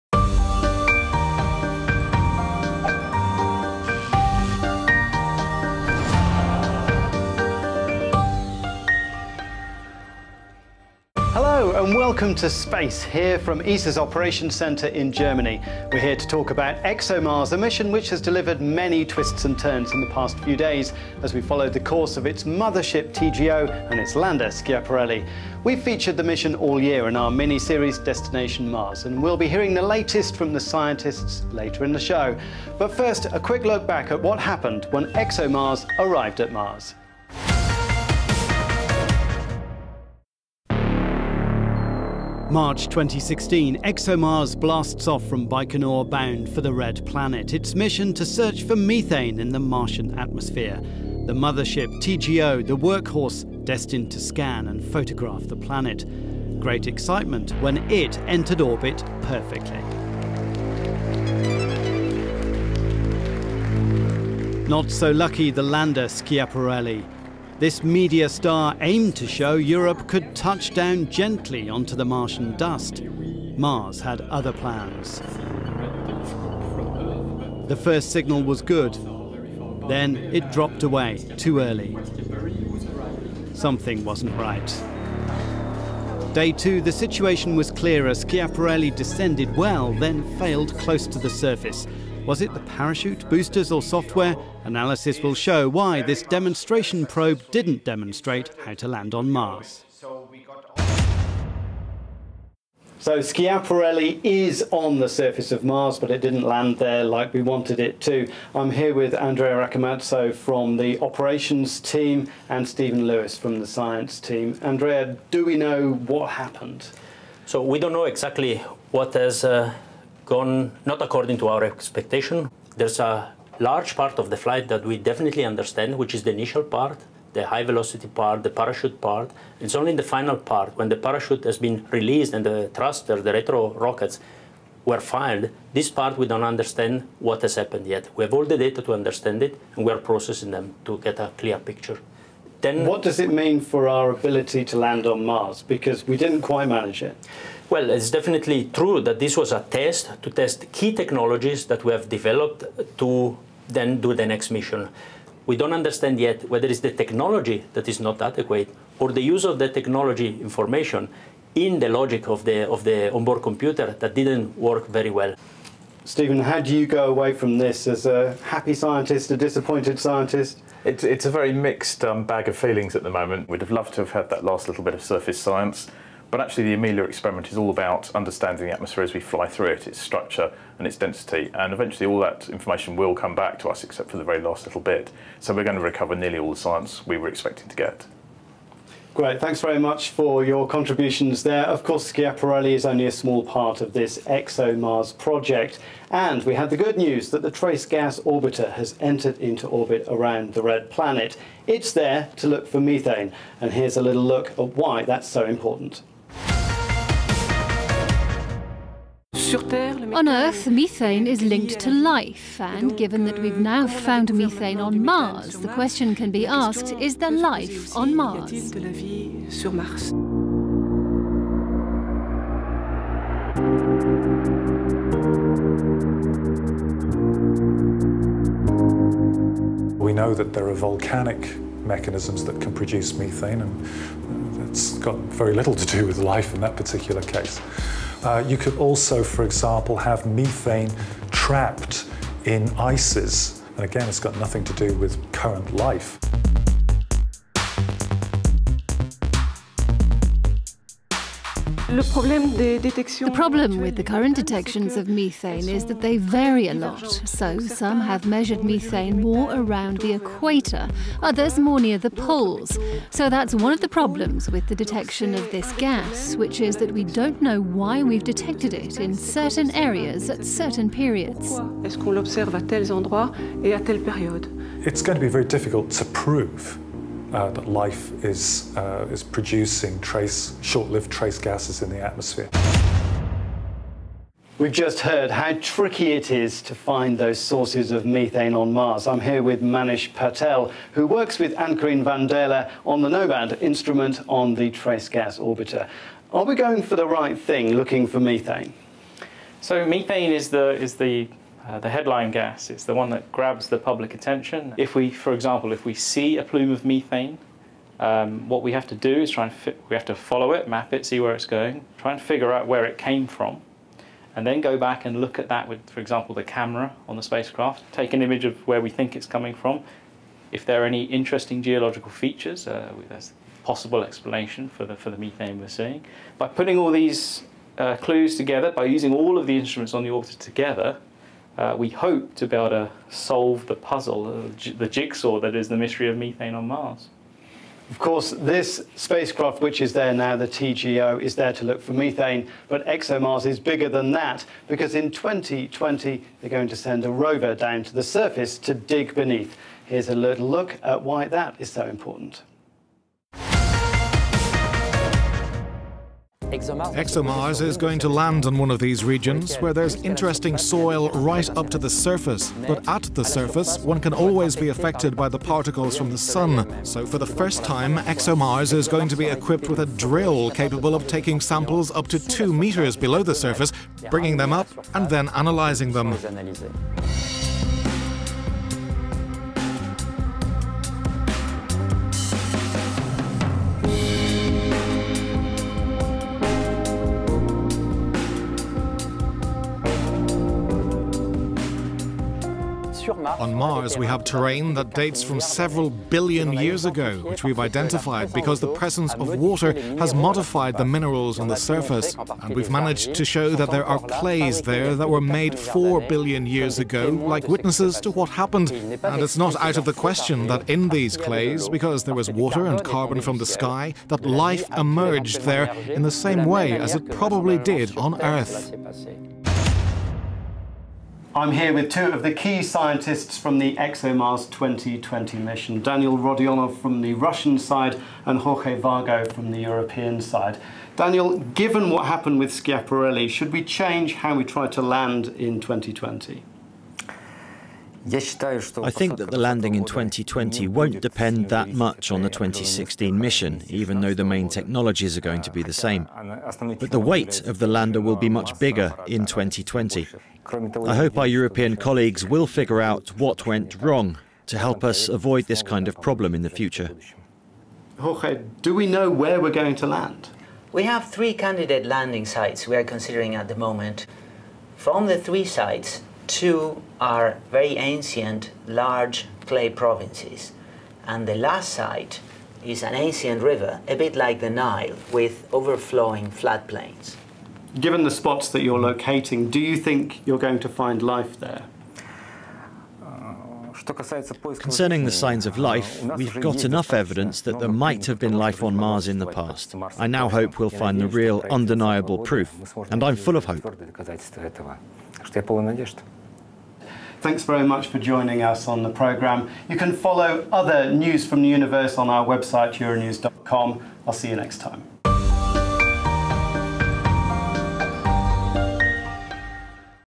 ESA Euronews: ExoMars at Mars
Scientists and engineers on the ExoMars project had their hearts in their mouths as the ExoMars mission reached the red planet, with the Schiaparelli probe going missing in action at the end of its descent just as the TGO mothership swept into a perfectly timed orbit.

The rollercoaster ride of arrival at Mars is the first installment in this ambitious Russian and European project that aims for the first time to directly search for signs of life on Mars.

The plight of Schiaparelli remains unclear. It is certainly on the Martian surface, but may well have hit the red dust much harder then engineers had planned, and nothing has been heard from it since.

Data relayed during the lander's descent shows the initial high-speed entry to the Martian atmosphere went well, with the heatshield slowing the craft and the parachute deploying. However once the back heat shield and parachute were ejected the flow of events did not go to plan.